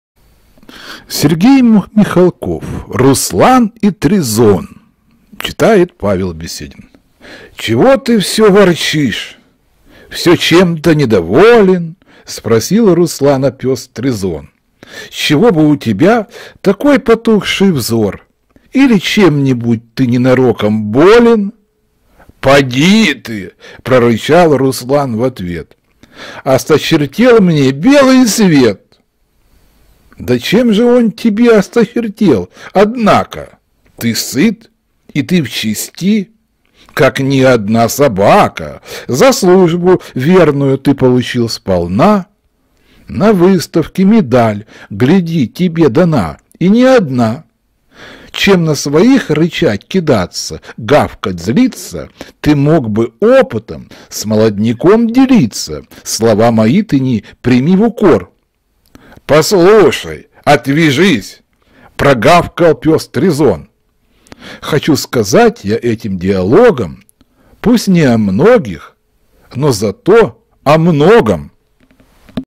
Руслан и Трезон Сергей Михалков читает Павел Беседин
«Чего ты всё ворчишь — всё чем-то недоволен?! —
Спросил Руслана пёс Трезор. —
С чего бы у тебя такой потухший взор?
Иль чем-нибудь ты ненароком болен?» —
«Поди ты… — прорычал Руслан в ответ. —
Осточертел мне белый свет!» —
«Да чем же он тебе осточертел, однако?
Ты сыт, и ты в чести, как ни одна собака, —
За службу верную ты получил сполна,
На выставке медаль, гляди, тебе дана — и не одна!
Чем на своих рычать, кидаться, гавкать, злиться,
Ты мог бы опытом с молодняком делиться.
Слова мои ты не прими в укор!» —
Поставил точку смелый пёс Трезор.
Хочу сказать я этим диалогом
Пусть не о многих, но зато о многом!